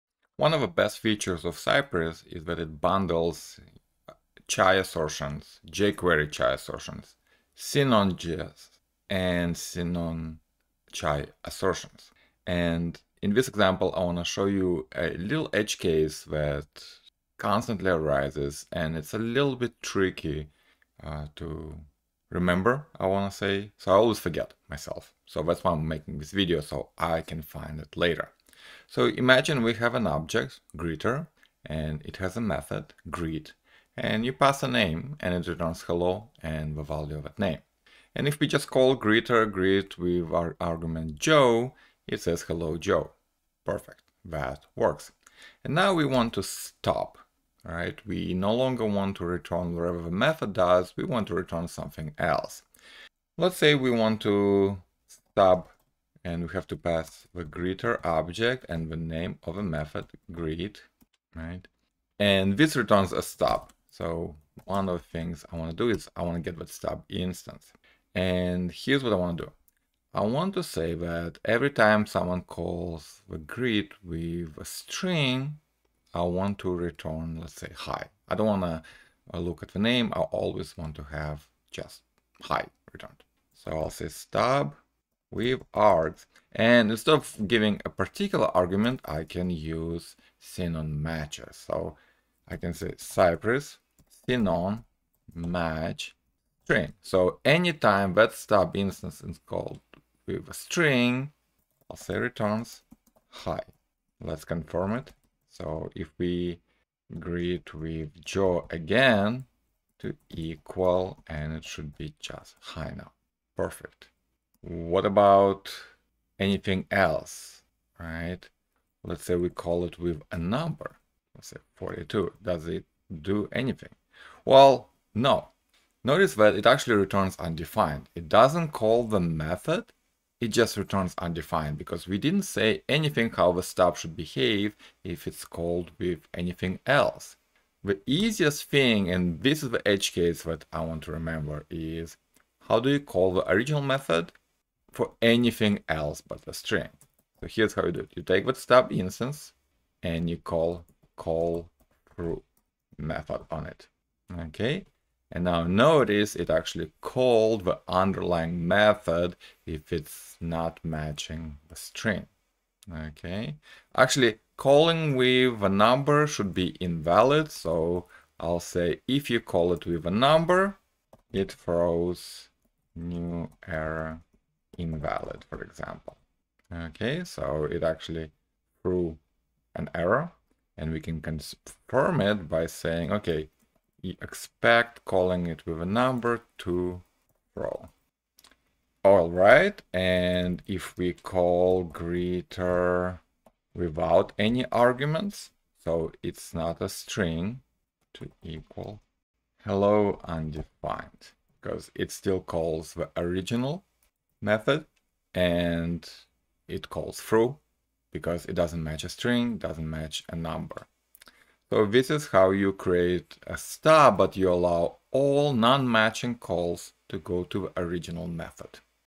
Stub A Method Depending On The Call Arguments
If we want to stub a method, we can use the Cypress command cy.stub, which creates a Sinon.js stub. We can refine the stub by telling it what to do depending on the argument values and types. We can even call the original method for every non-specified variant

const greeter = {
   * Greets a person
   * @param {string} name
  greet(name) {
    return `Hello, ${name}!`

const stub = cy.stub(greeter, 'greet')
// all non-matched calls should call the real method
stub.callThrough()
// all calls with a string argument should get "Hi"
stub.withArgs(Cypress.sinon.match.string).returns('Hi')
// all calls with a number argument should raise an error
stub
  .withArgs(Cypress.sinon.match.number)
  .throws(new Error('Invalid name'))
// let's confirm the stub is working
// case 1: if we call it with a string argument
// it returns "Hi"
expect(greeter.greet('World')).to.equal('Hi')
// case 2: if we call it with a number argument
// it raises an error
expect(function () { greeter.greet(42) }).to.throw('Invalid name')
expect(greeter.greet).to.have.been.calledTwice

// case 3: non-matched calls invoke the actual method
expect(greeter.greet()).to.equal('Hello, undefined!')